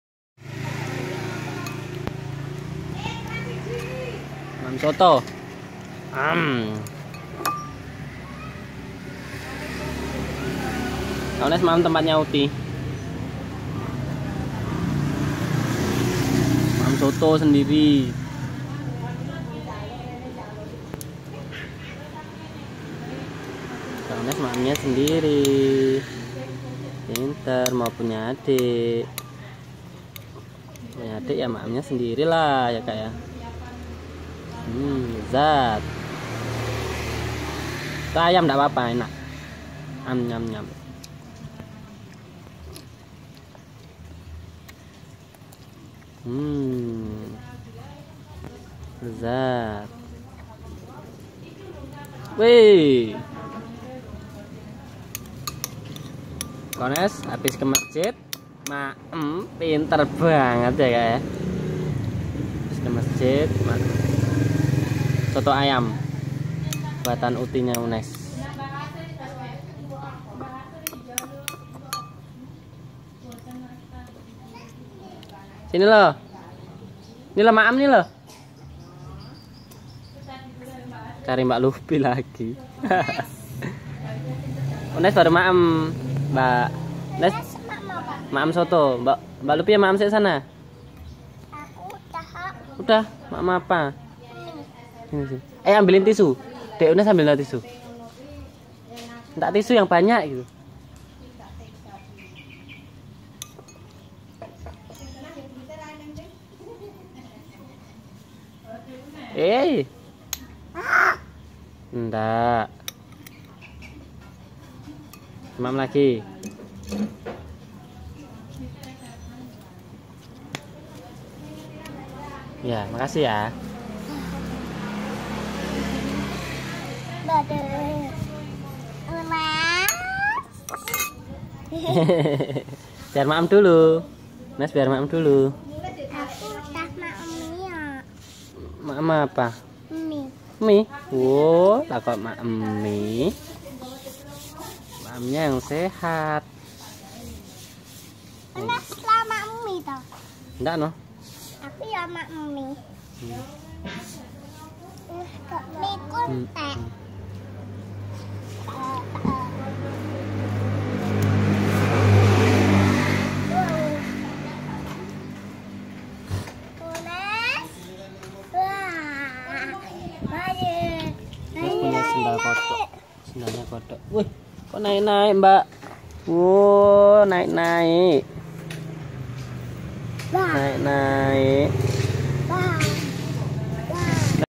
Anak kecil Makan Sendiri pakai Soto buatan Uti - kakaunais
Anak kecil makan Soto sendiri
Belajar Makan Sendiri
Anak Sehat
Anak Cerdas
Anak Pintar

Permainan Asyik bersama KakaUnais